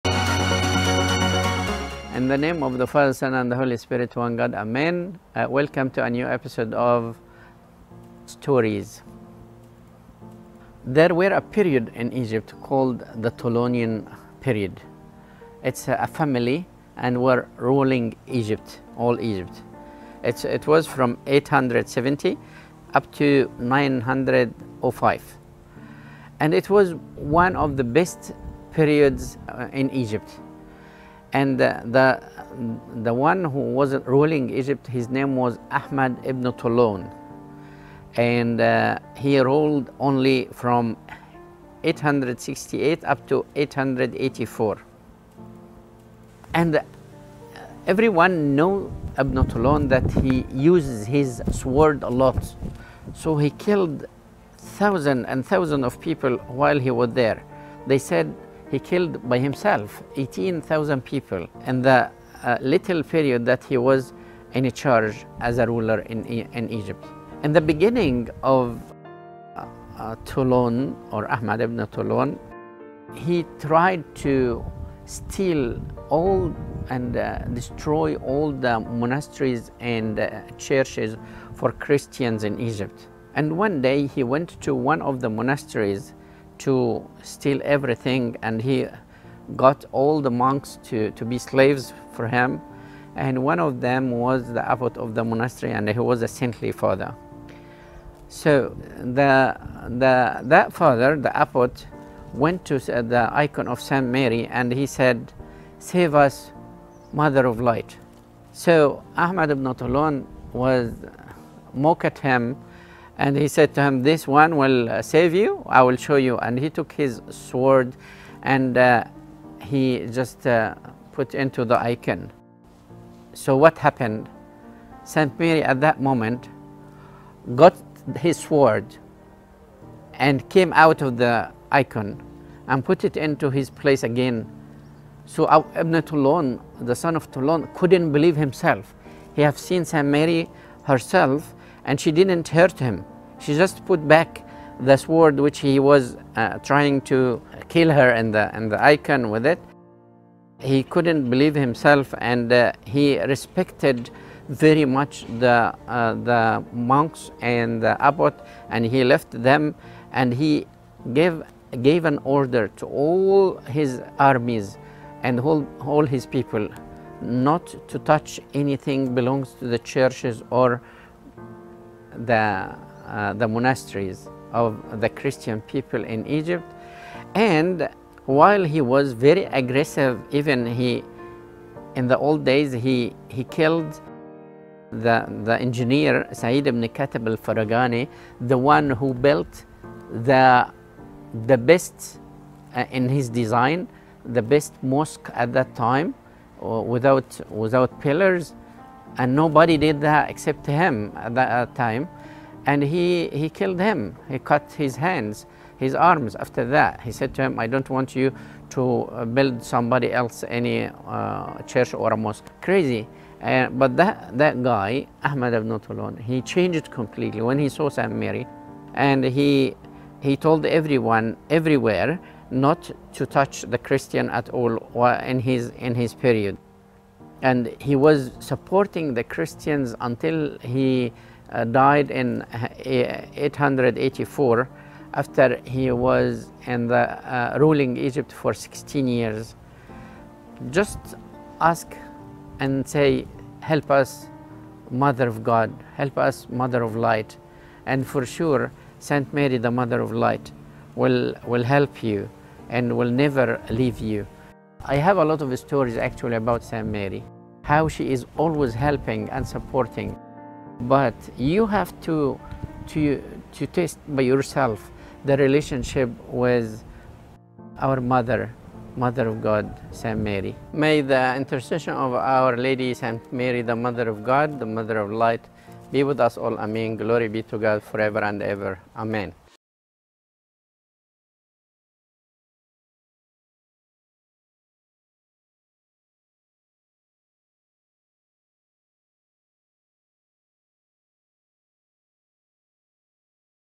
Stories S06 E02: Help Me, Mother of Light - CYC
There is a period in Egypt that called the Toulonien Period,
Egypt was ruled by " Ahmed Ebn Toulon", He wanted to destroy all the Christian's monasteries and Churches in Egypt. but St. Mary didn't stand still! .What did she do?!
What is the Miracle that St. Mary did back in That Time?

Lets hear the Story from the mouth of our beloved  Fr. Bishoy El Antony